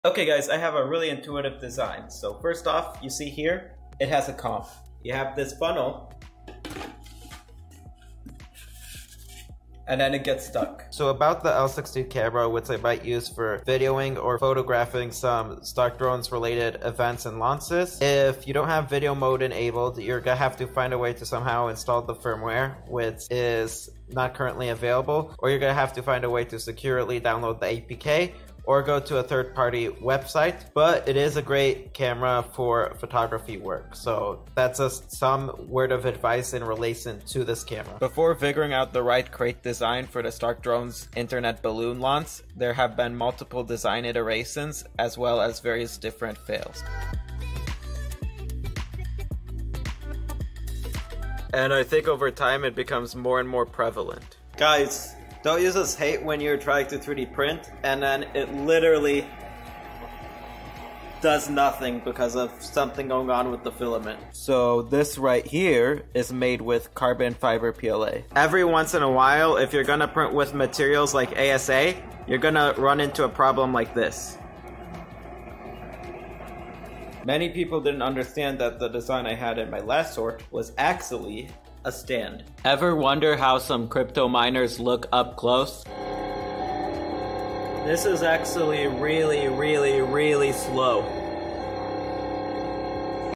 Stark Drones Corporation: Some of its Recent Shorts (Including Some 3D Printing Clips)
The latest short series for the Stark Drones YouTube channel all in a single reel.

Video 1:
Okay guys, I have a really intuitive design, so first off, you see here, it has a comb, you have this funnel, and then it gets stuck.

Video 2:
So about the L16 Camera which I might use for videoing or photographing some Stark Drones related events and launches, if you don't have video-mode enabled, you are going to have to find a way to somehow install the firmware which is not currently available, or you are going to have to find a way to securely download an APK, or go to a third party website, but it is a great camera for photography work, so that is just some word of advise in relation to this camera.

Video 3:
Before figuring out the right crate design for the Stark Drones internet balloon launch, there have been multiple design iterations as well as various different fails. and I think over time it becomes more and more prevalent.

Video 4:
Guys, don't you just hate when you try to 3D print and then it literally does nothing because of something going on with the filament?

Video 5:
So this right here, is made with Carbon Fiber PLA!

Video 6:
Every once and a while if you are gonna print with materials like ASA, you are gonna run into a problem like this.

Video 7:
Many people didn't understand that the design I had in my last short was actually a stand.

Video 8:
Ever wonder how some crypto miners look up close?

Video 9:
This is actually really, really, really slow!